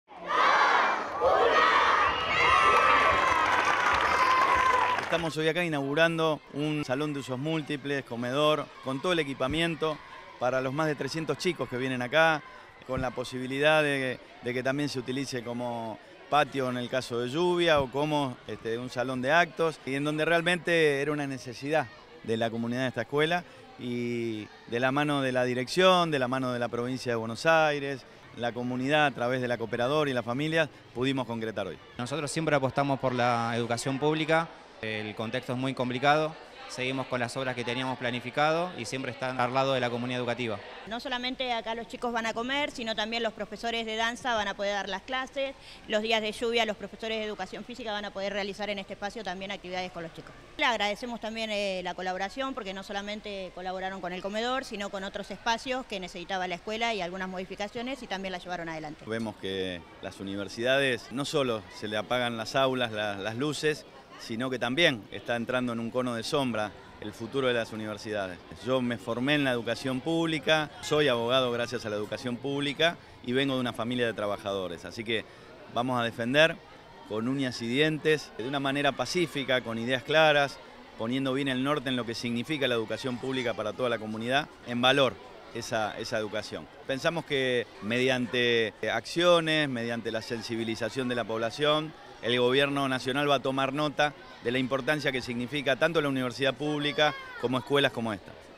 Julio Zamora: “Vengo de una familia de trabajadores y soy abogado gracias a la educación pública»
Lo destacó el intendente de Tigre en el marco de la inauguración del comedor -y SUM- de la Escuela Primaria N° 36 de General Pacheco. El espacio construido por el Municipio, será utilizado por más de 170 alumnos y cuenta con nuevo mobiliario de última generación y una superficie cubierta que abarca 300 m2.